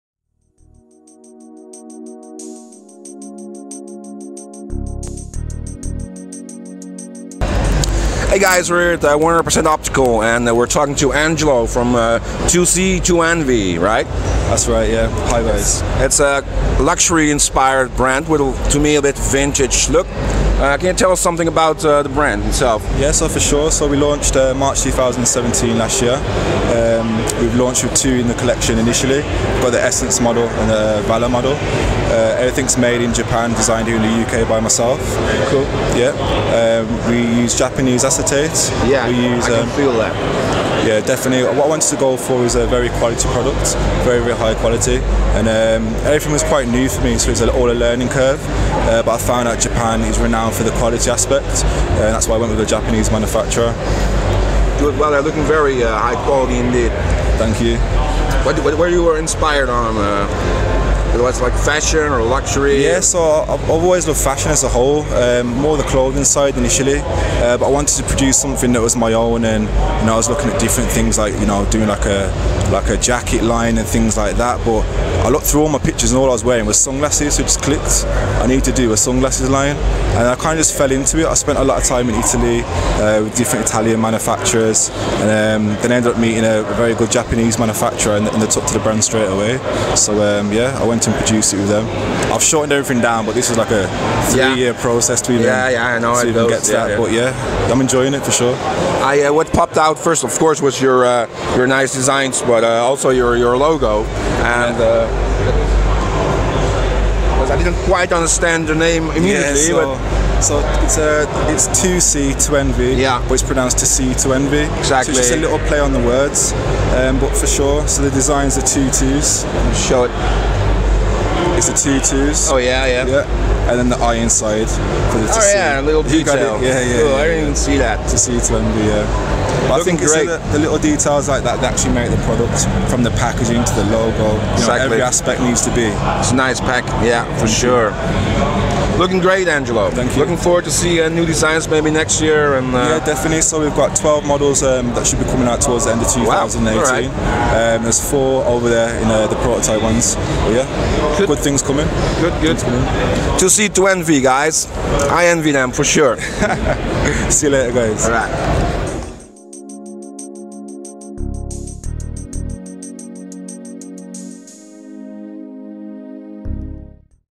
The Best New British Designer Sunglasses 2018 with The Eyewear Forum
100% Optical at the London ExCel hosted an incredible array of indie eyewear. Amongst the fresh direction, new British eyewear designer, Angelo Luiliani, speaks with Maarten from The Eyewear Forum, about his inspirations and the origins of his brand 2C2NV - To See To Envy.

You can read more about 2C2NV and other new British based indie eyewear in our blog linked below: